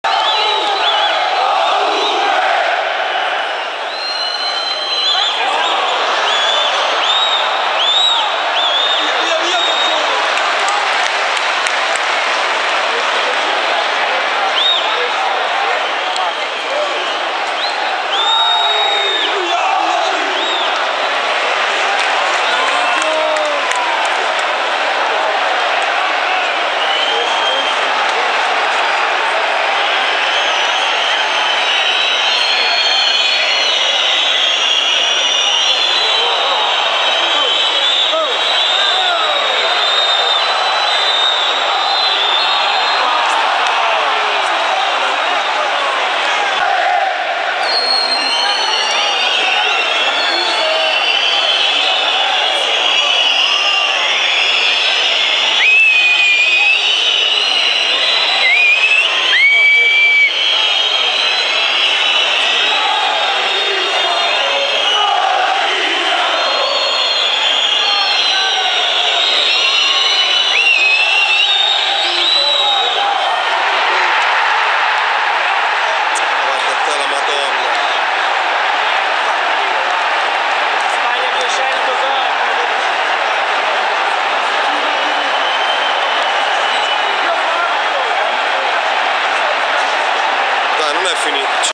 JUVENTUS Vs Manchester United Goal Juan Mata 1-1
Champions League Girone H 4^ giornata  86' 2° tempo JUVENTUS Stadium Torino 07/11/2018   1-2(Cristiano,J.Mata,Alex Sandro[Aut.])